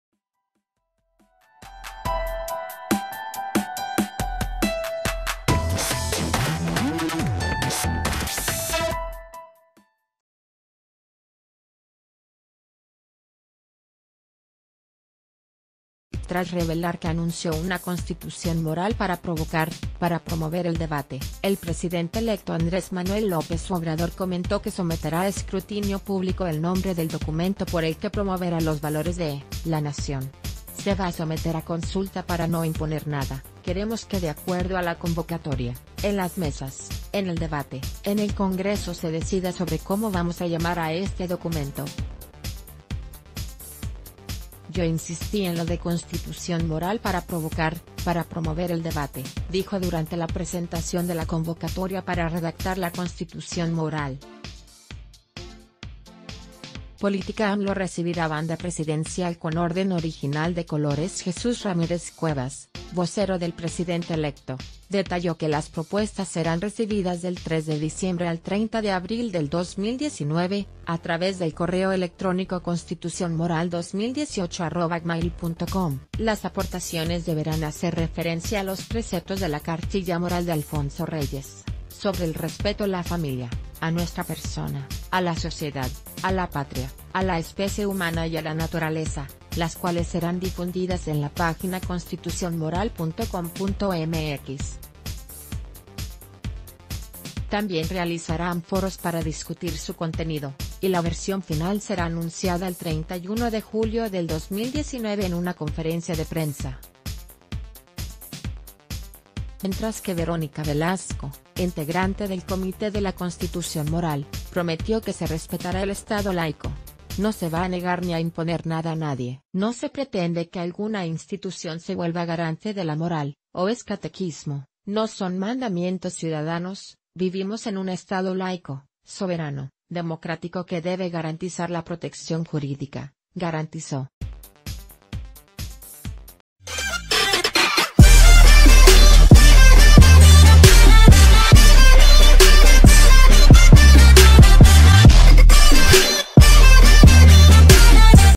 AMLO recibirá propuestas ciudadanas para crear la Constitución Moral
Tras revelar que anunció una "constitución moral para provocar, para promover el debate", el presidente electo Andrés Manuel López Obrador comentó que someterá a escrutinio público el nombre del documento por el que promoverá los valores de la nación."Se va someter a consulta para no imponer nada, q...